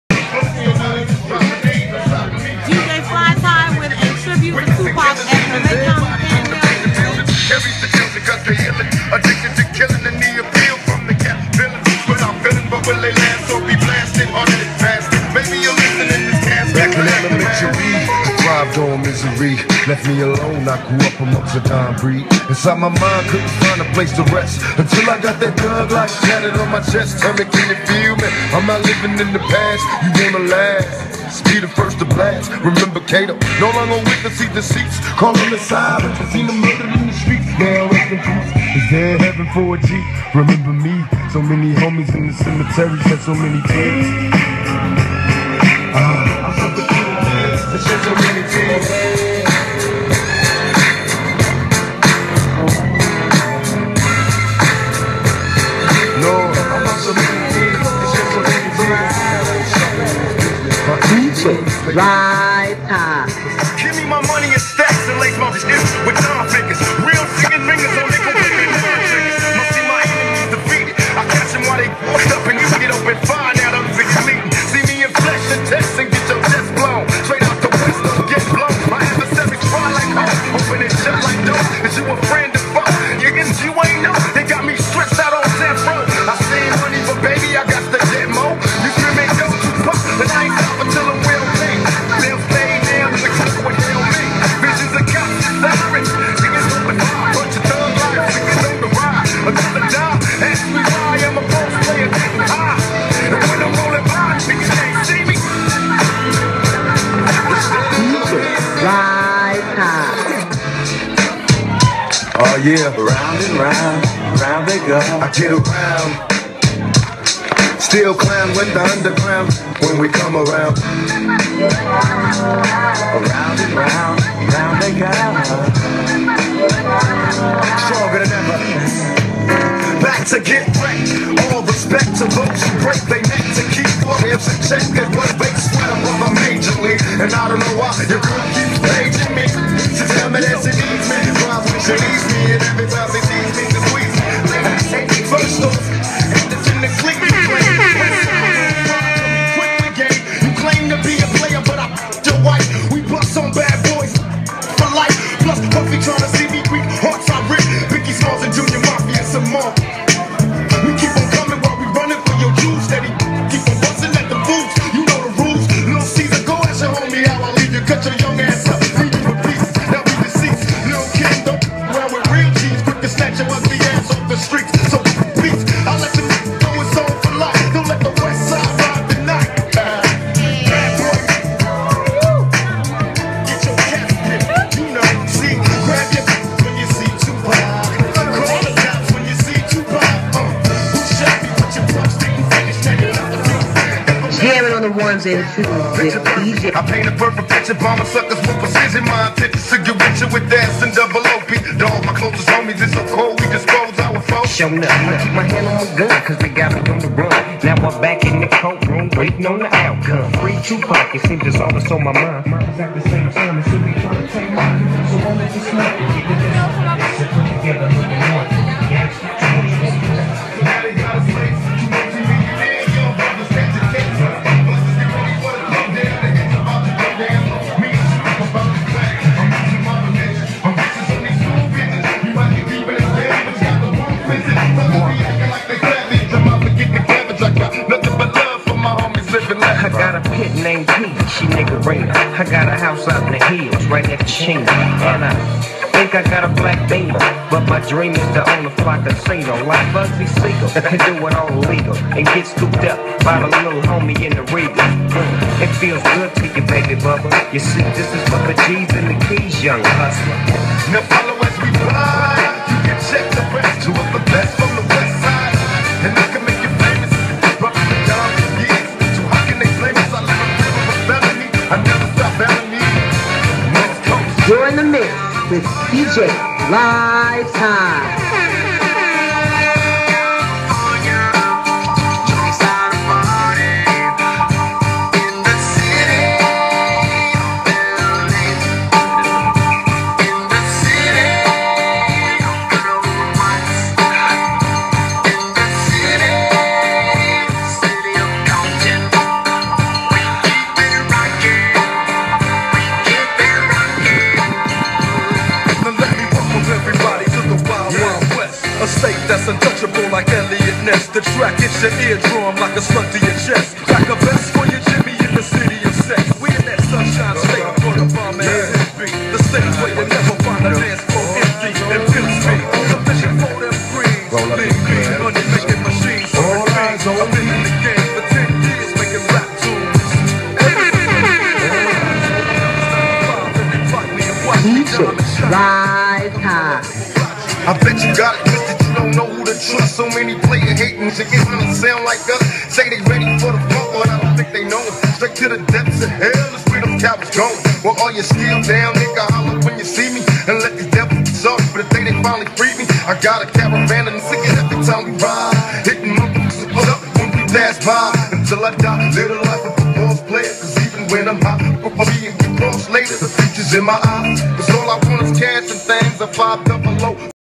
DJ Fly Ty LIVE Tupac Tribute June 18, 2016
DJ Fly Ty is a 13 year old DJ and loves working with music.